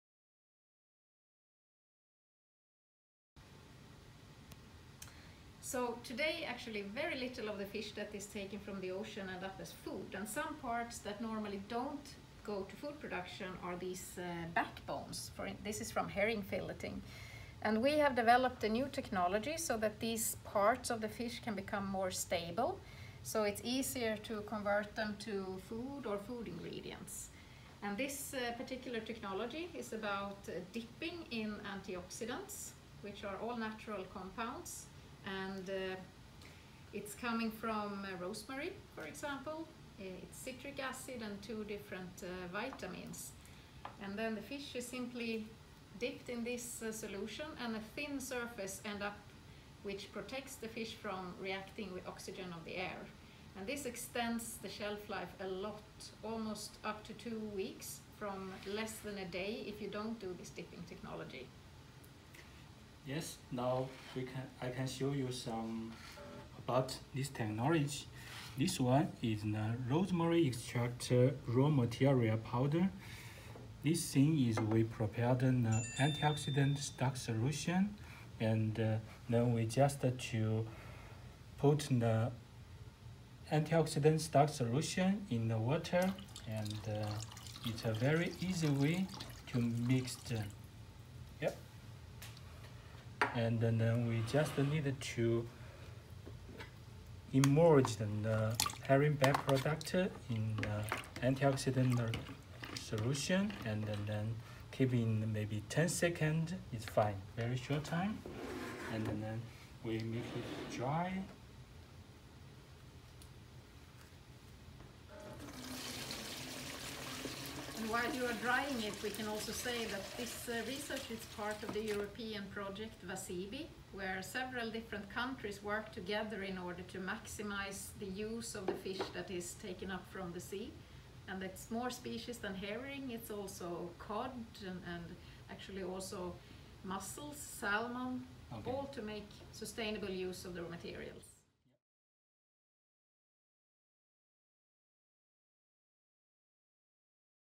How does the new dipping technology work?
New dipping solution turns the whole fish into valuable food

When herring are filleted, more than half their weight becomes a low-value ‘side stream’ that never reaches our plates – despite being rich in protein and healthy omega-3 fatty acids.

Now, scientists from Chalmers University of Technology, Sweden, within the frame of the project WaSeaBi, have developed a special dipping solution, with ingredients including rosemary extract and citric acid, which can significantly extend the side streams’ shelf life, and increase the opportunities to use them as food.

The study, Controlling hemoglobin-mediated lipid oxidation in herring (Clupea harengus) co-products via incubation or dipping in a recyclable antioxidant solution, was published with open access in the journal Food Control.

Film clip by kind permission: Mia Halleröd Palmgren/Chalmers University of Technology